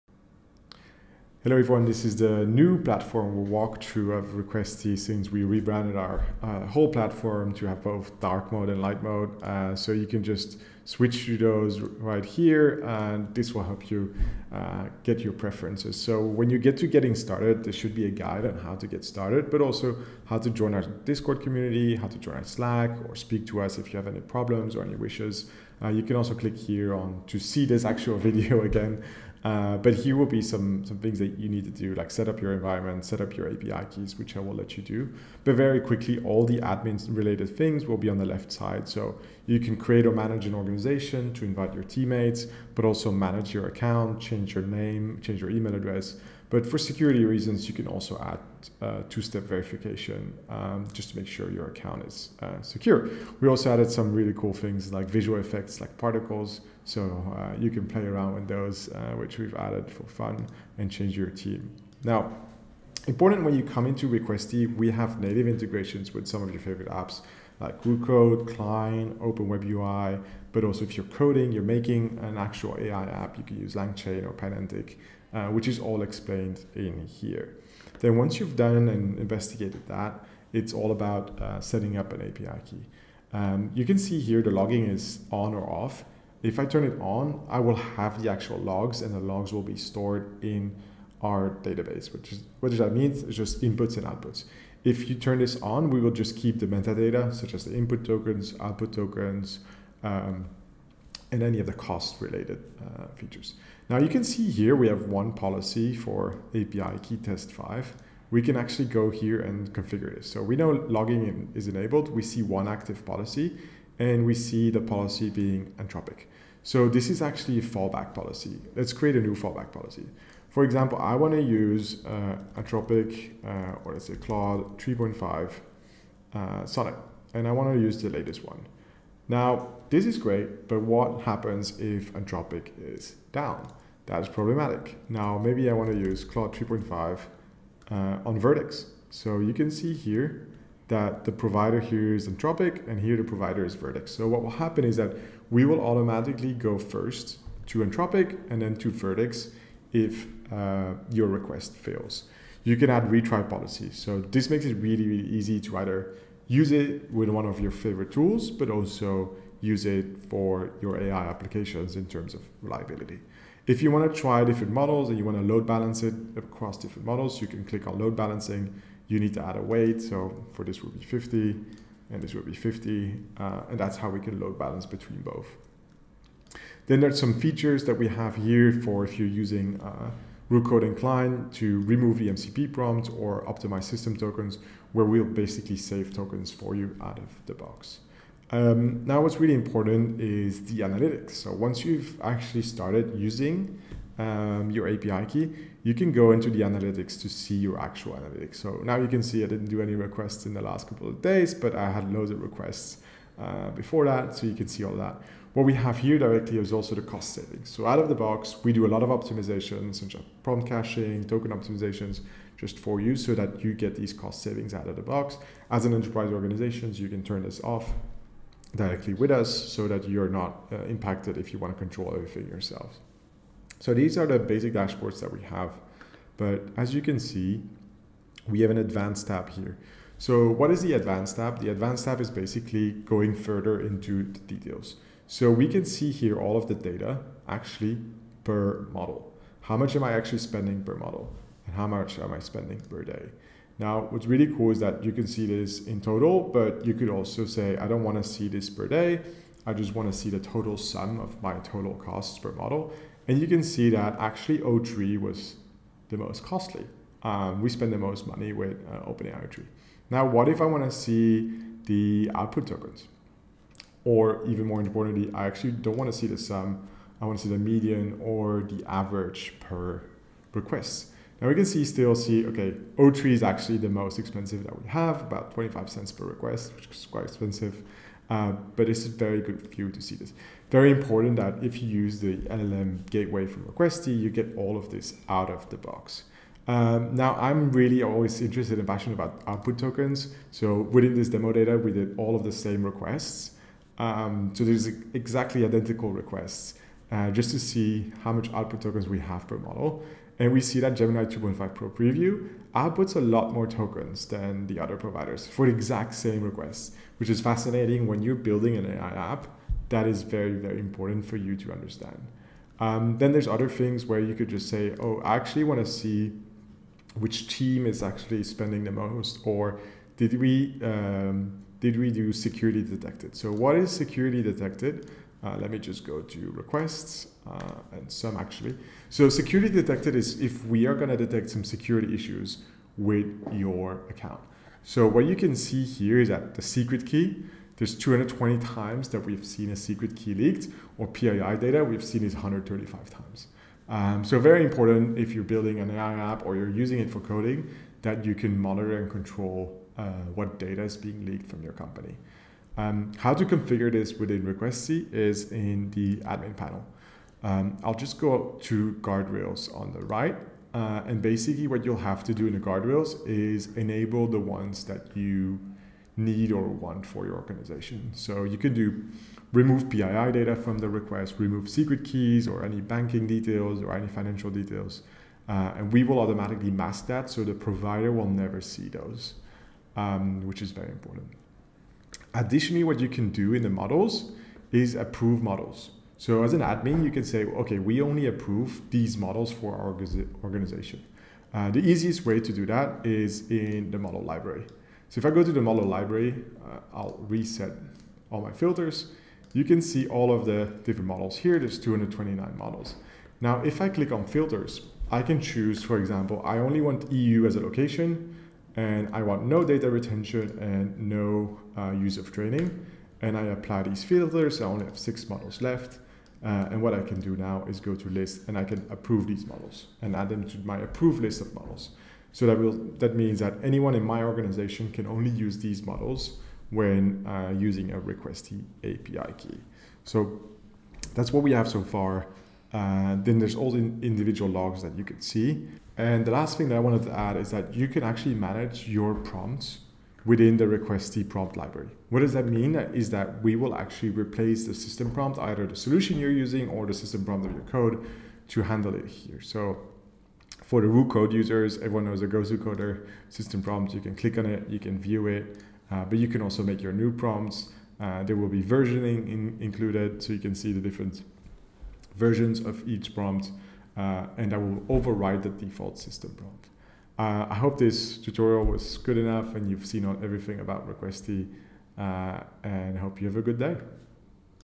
Requesty Platform 2.0
One Gateway. Every LLM. Total Control.

Stop juggling APIs and providers. Requesty gives you a single, powerful interface to access any LLM—OpenAI, Anthropic, Mistral, Groq, and more.

✅ Centralized logging & analytics
✅ Built-in function calling & security
✅ One API. Unlimited flexibility.